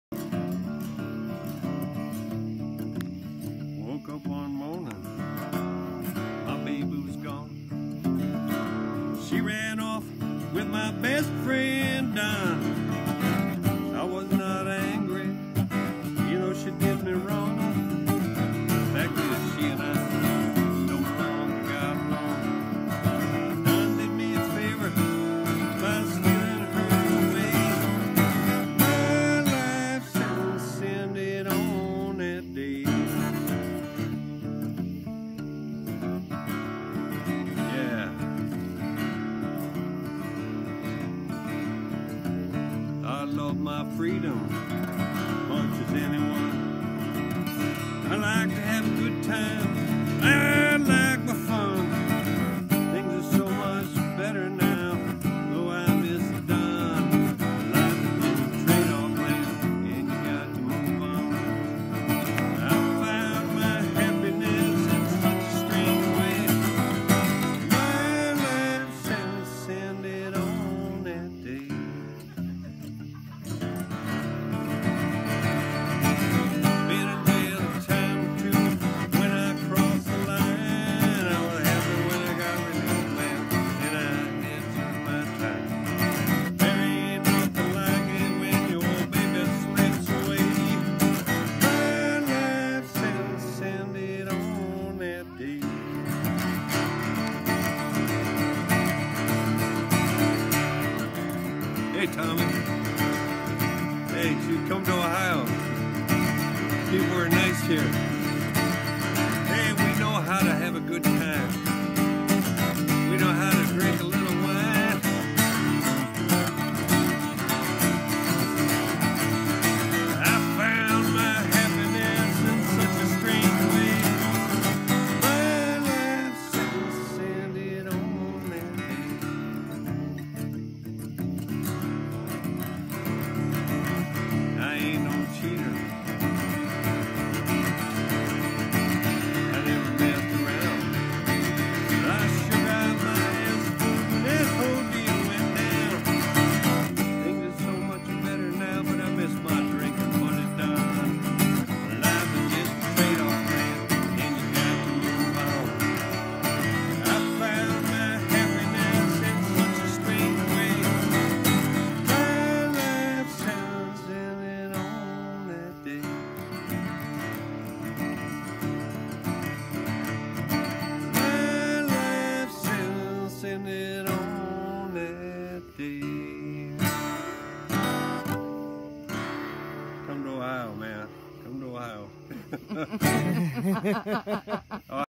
For Tommy Sweden from Skip Werke
Sending a song to Tommy Sweden Haglund from Dee Dee and Mark’s backyard. Sung by Skip Werke (Tom Chernitsky)